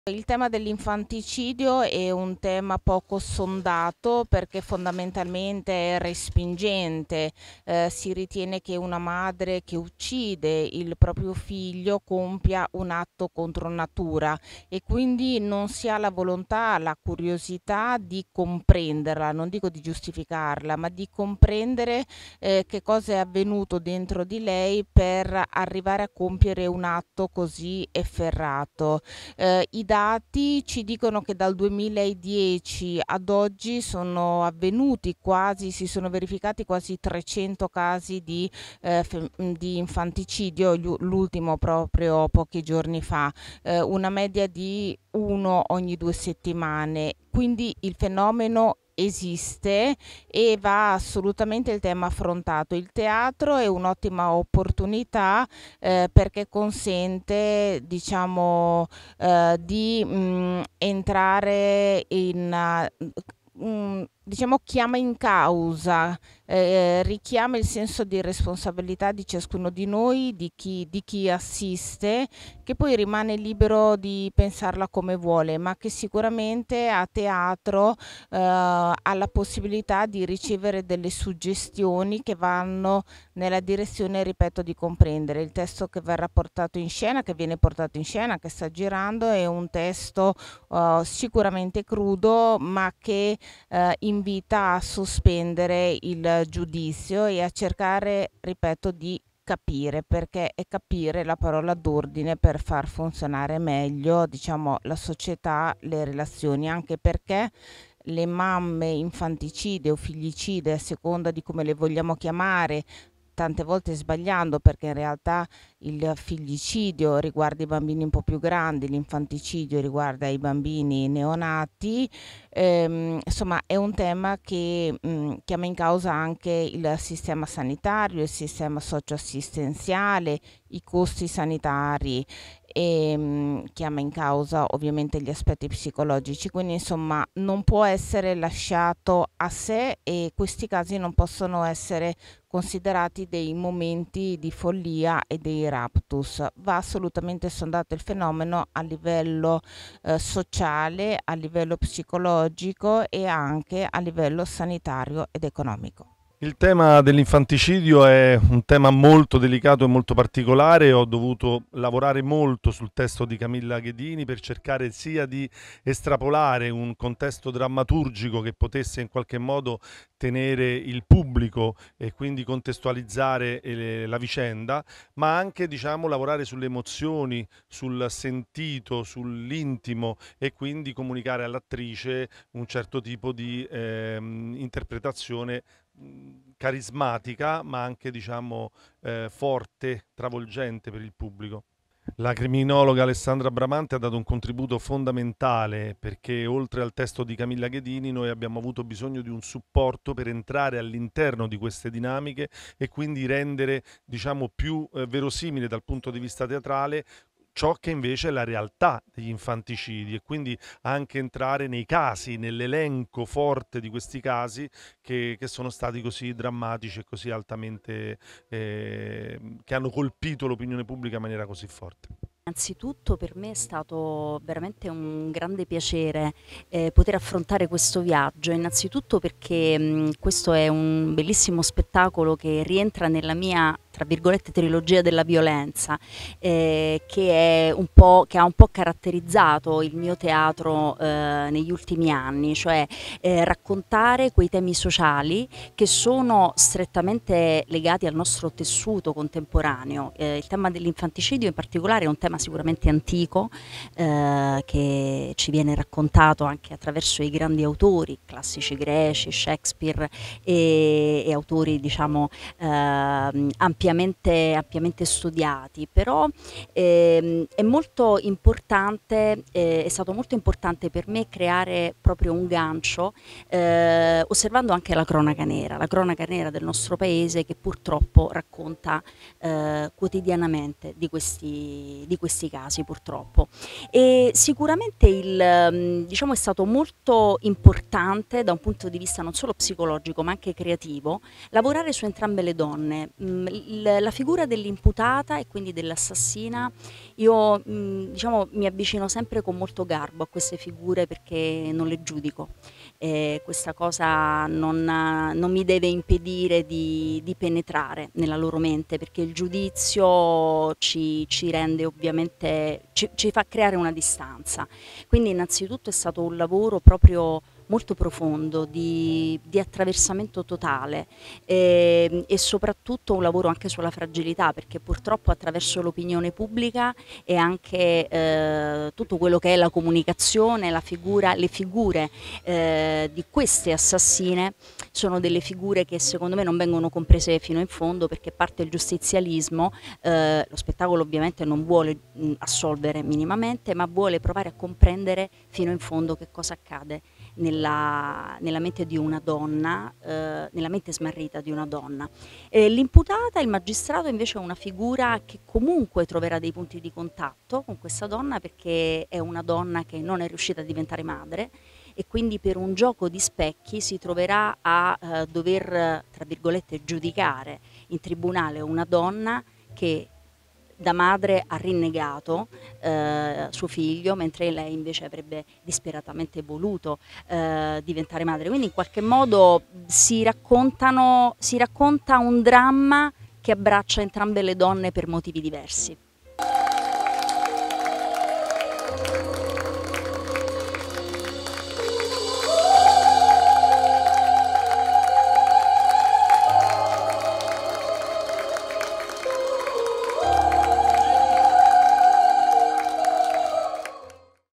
"Interruzioni - Le Crepe Dell'anima"
Testo di Camilla Ghedini con Carmen Di Marzo regia di Paolo Vanacore
3° serata del Festival del Pensiero Cristallino dalla Planteria Orto botanico di Locri (RC)